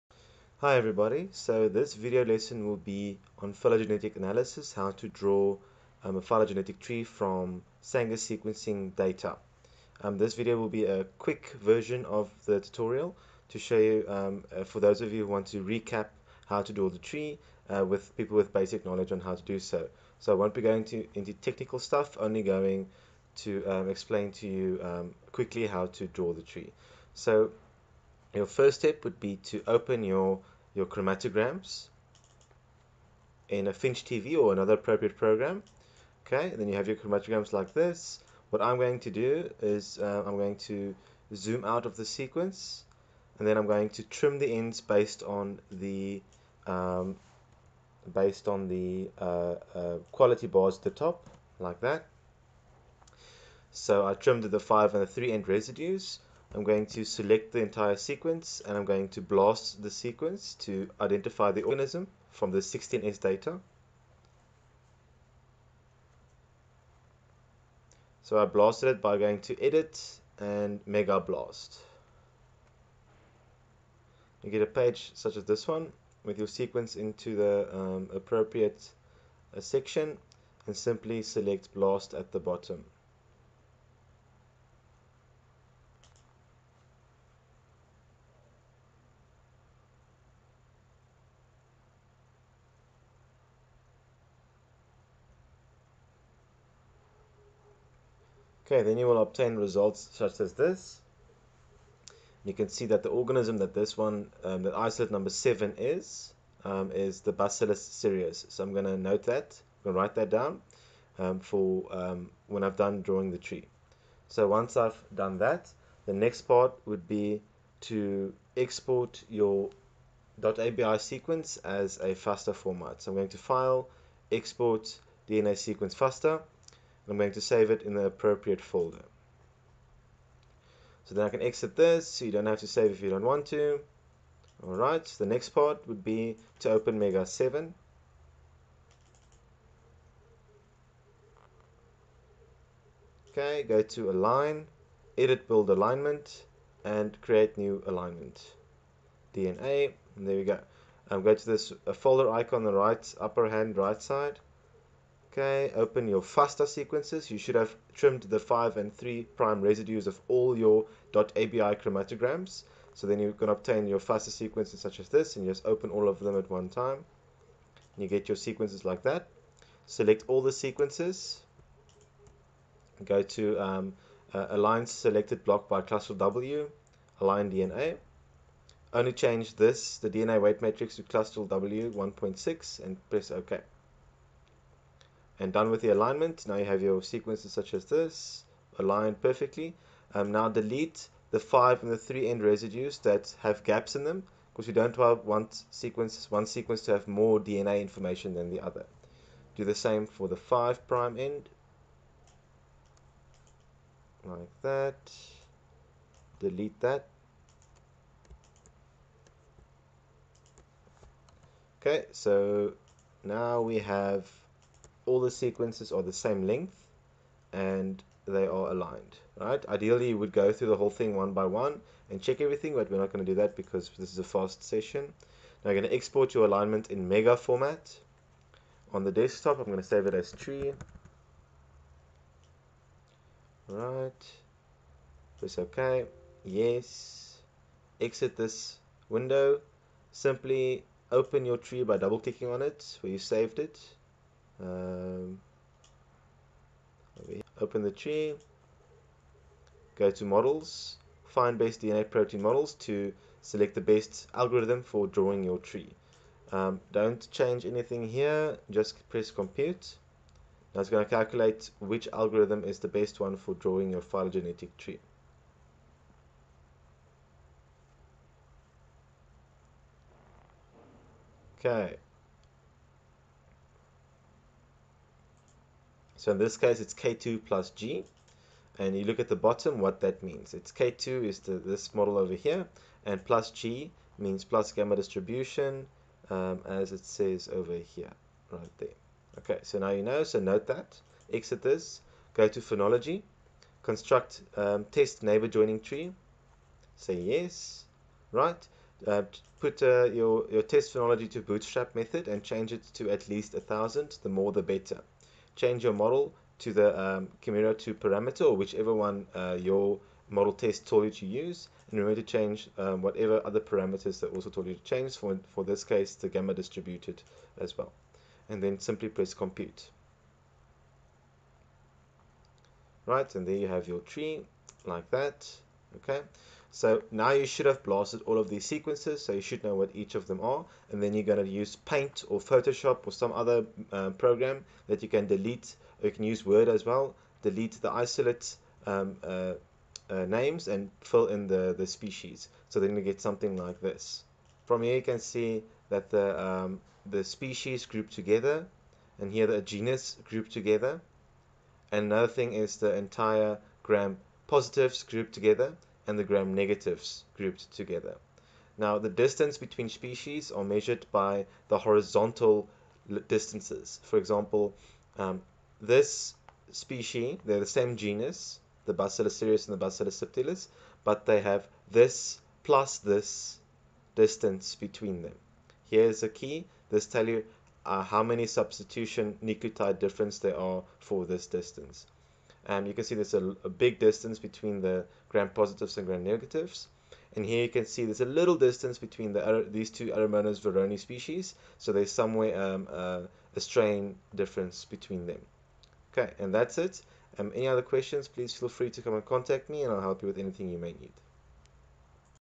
How to draw a phylogenetic tree with Sanger sequencing data
This video is intended for people who have an idea how phylogenetic construction works, or who just want a "quick fix". If you follow this process you will be able to draw a quick tree in a matter of minutes. Programs used are FinchTV and MEGA 7.